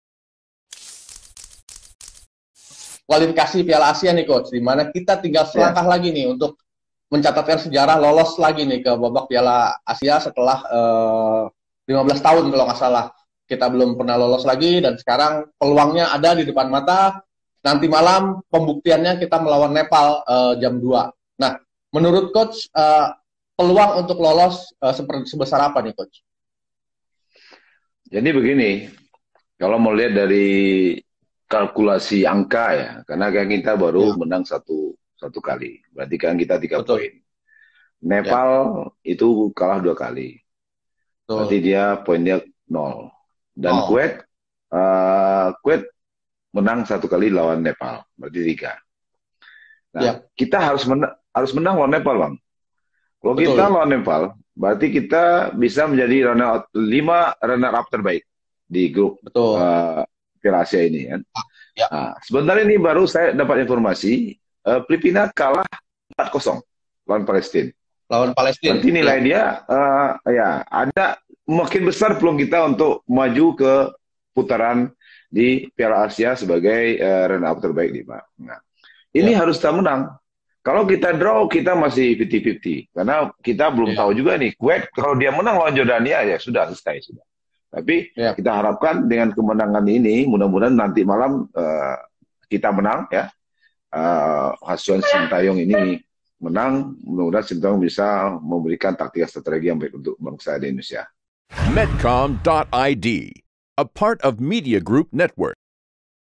Jelang Laga Pamungkas Indonesia Vs Nepal, Nil Maizar Indonesia Harus Menang
Mantan Pelatih Timnas, Nil Maizar, mengatakan bahwa Timnas Indonesia harus menang melawan Nepal. Sehingga Indonesia memiliki peluang besar untuk tampil di Piala Asia 2023 tahun depan.

Tak hanya itu, Timnas Indonesia juga memiliki peluang untuk menduduki posisi lima runner-up terbaik. Namun, menurutnya, Timnas Indonesia tidak memiliki peluang jika hasil pertandingan melawan Nepal seri, dan Kuwait menang atas Yordania.